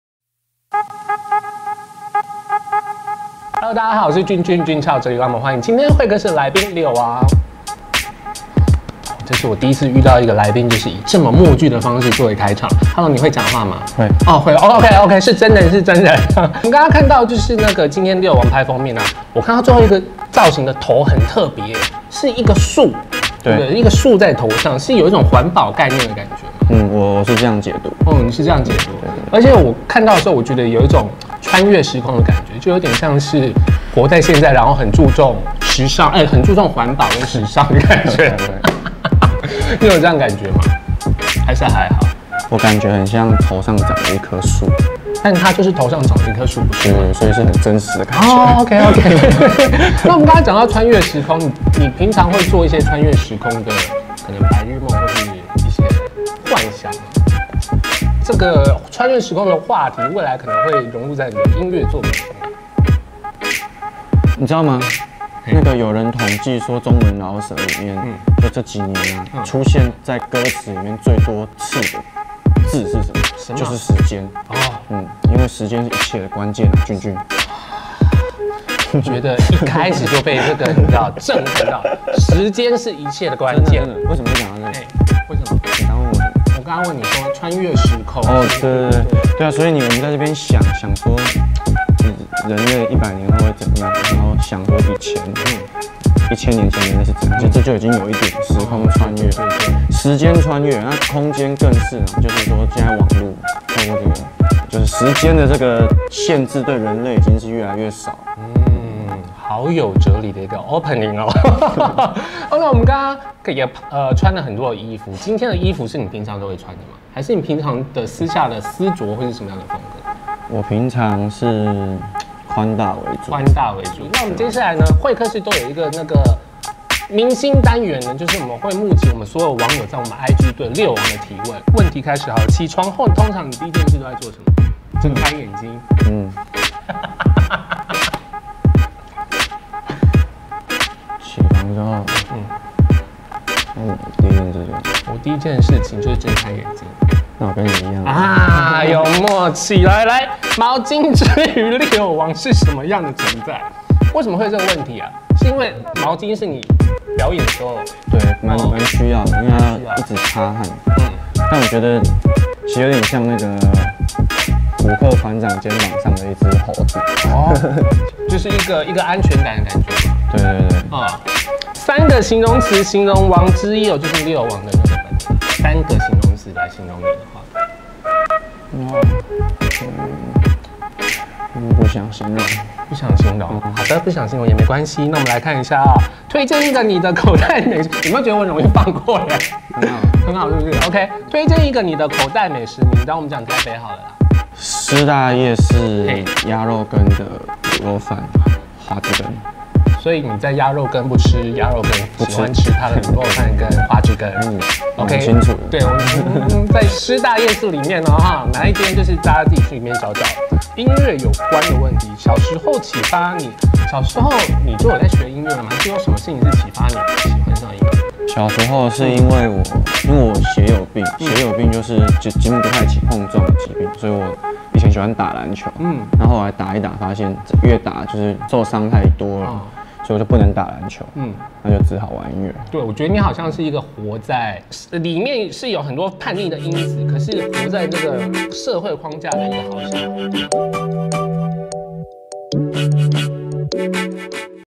MilkX會客室 | Leo王— 王式哲學・上集
你知道中文饒舌裡出現最多次的歌詞是什麼嗎？

短短四分鐘的談話中，Leo王從日常聊到哲理，富有想法的他每分每秒腦力不停運轉，以獨特的視角感知生活、解讀世界，在偌大的宇宙中，開創屬於自己的一條航道。

starring_leo wang
host_ jun chan
video photographer & video production_auhstudio
editor_ ballballchiu, nicole chung ,albee chen
presented by MilkX taiwan